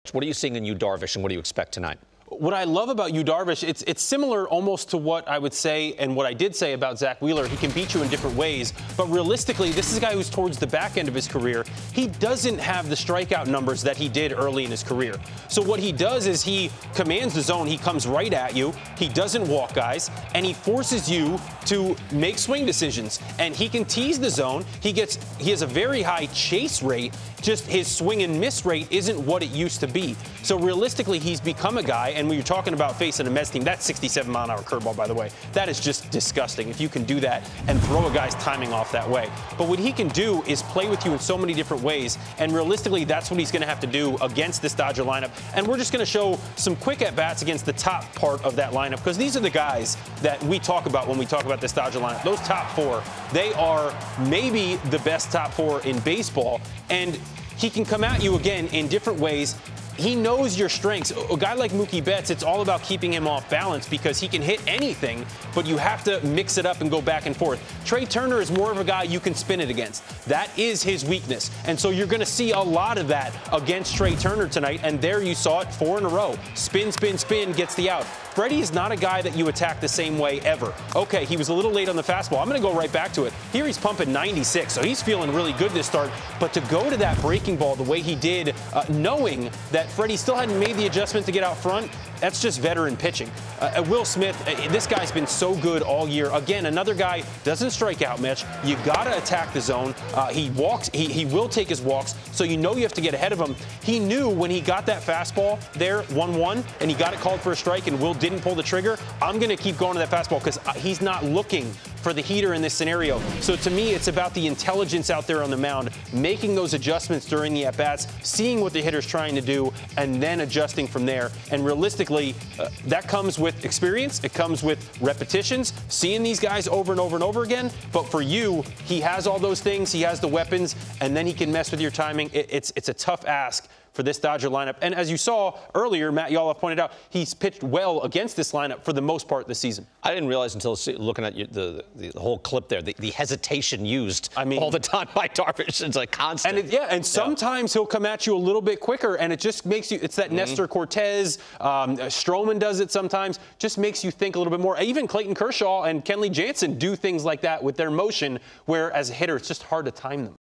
What Makes Yu Darvish So Effective?
Anthony Recker dives into the Padres' ace ahead of his Game 2 start vs Clayton Kershaw at Dodger Stadium.